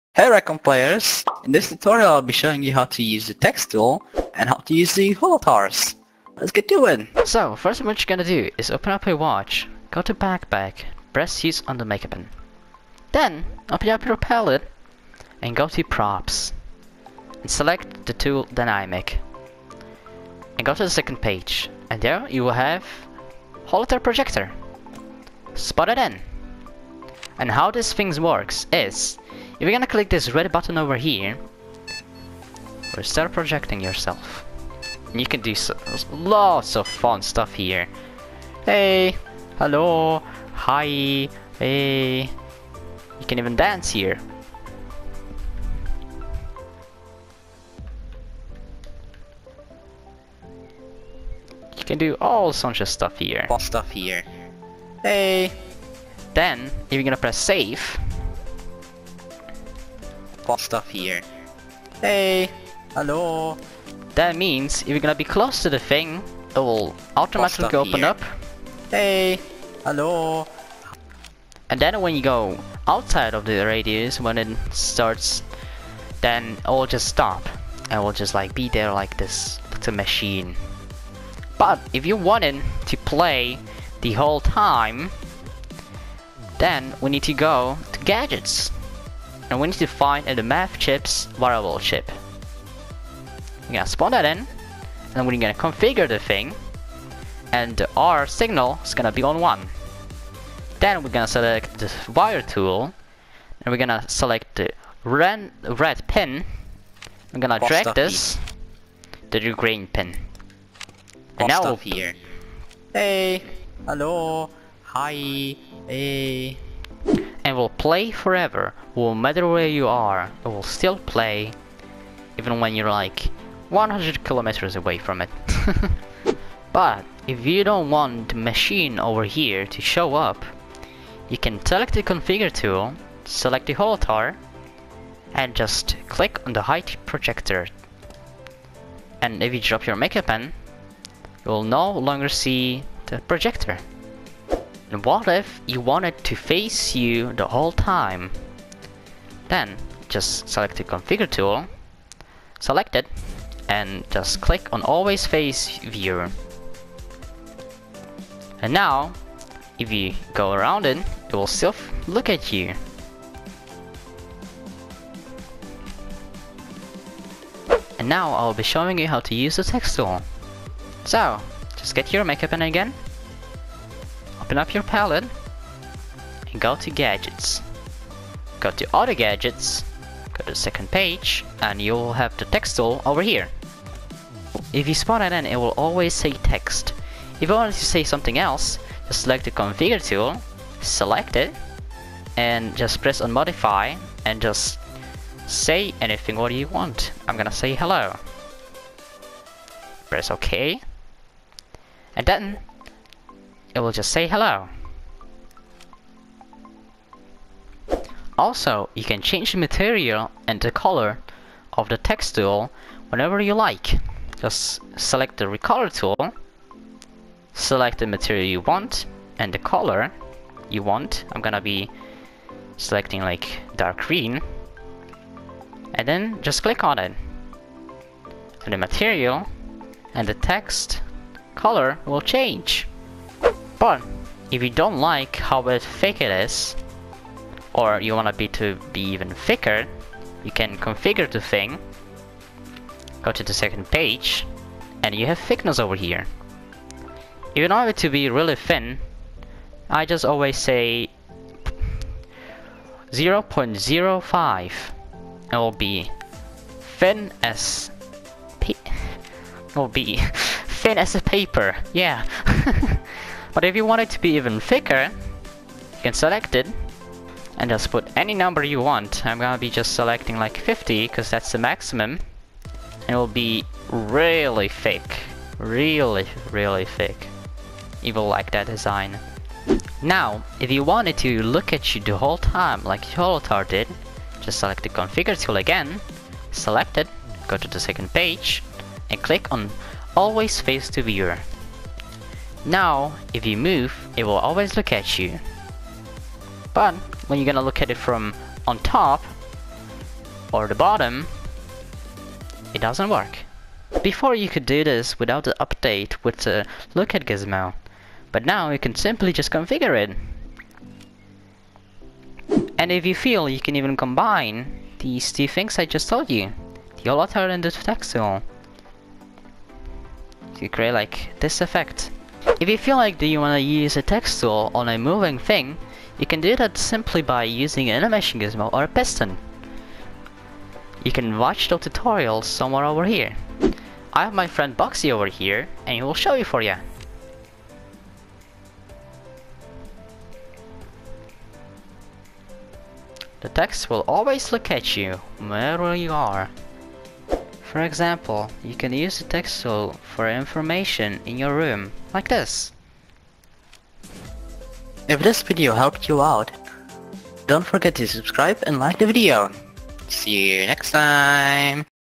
How to use the Text Tool and Holotars in RecRoom
In this video @Yellow_foxy will teach you how to use the Text Tool and Holotars!